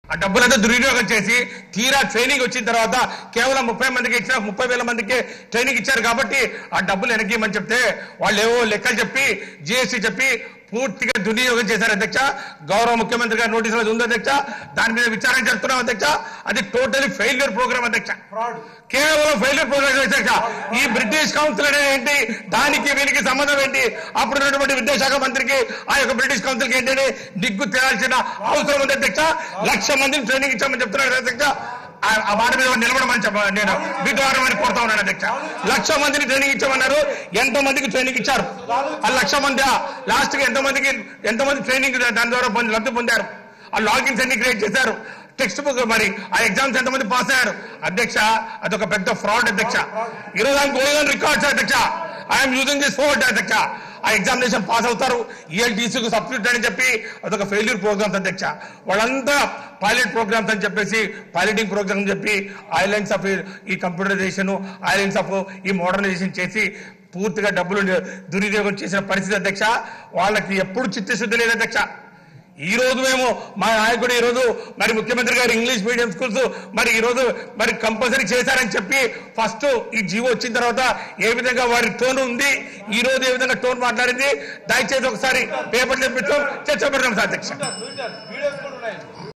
ఇంగ్లీష్ పై ట్రైనింగ్ పేరుతో నిధులు దుర్వినియోగం చేశారు - YCP Suresh - TV9
ఇంగ్లీష్ పై ట్రైనింగ్ పేరుతో నిధులు దుర్వినియోగం చేశారు - YCP Suresh